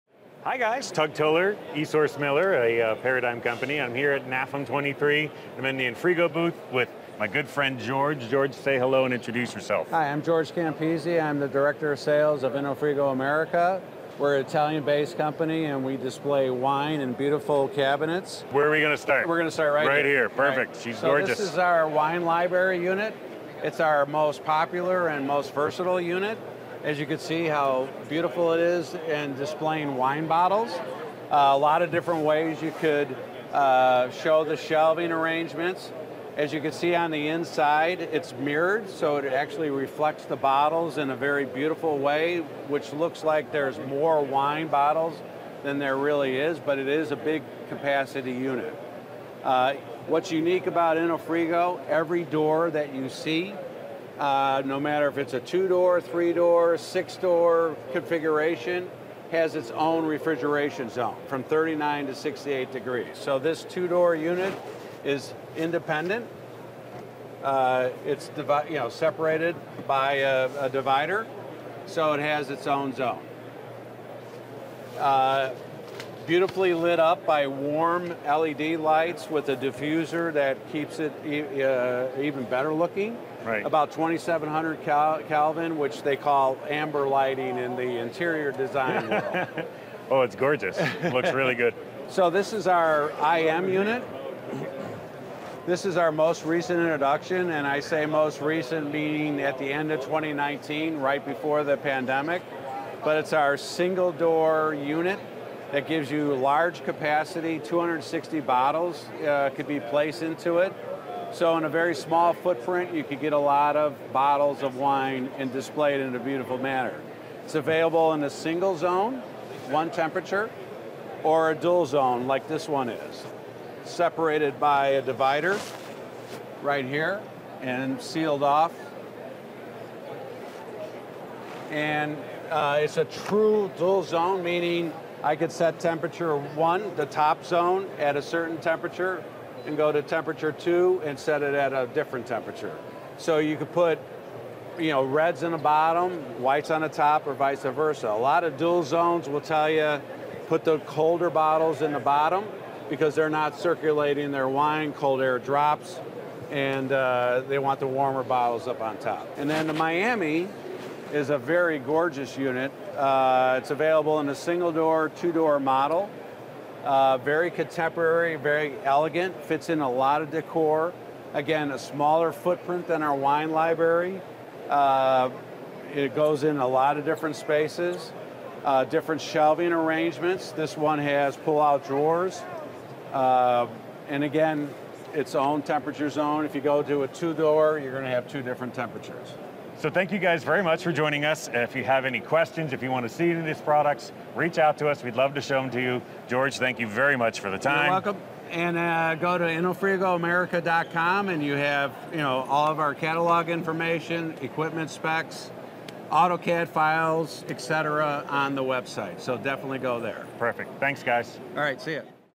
Enofrigo at NAFEM
Join Chef Tug Toler and George Campise in the Enofrigo booth at NAFEM as they showcase the line's impressive selection of gorgeous wine cabinets!